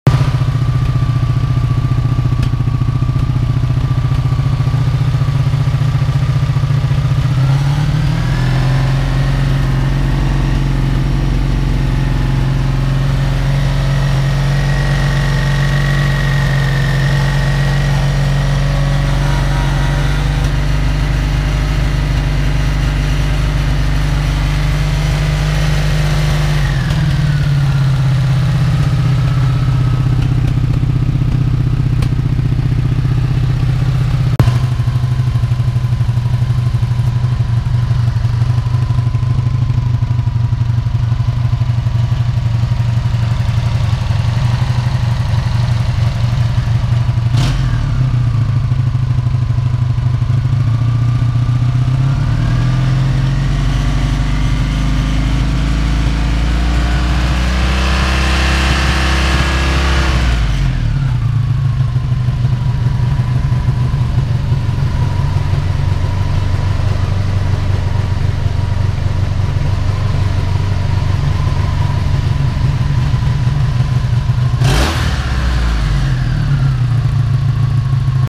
Before/After New Carb Boots - 83 Honda Magna V65
I finally broke down and got new carb boots for my Magna V65.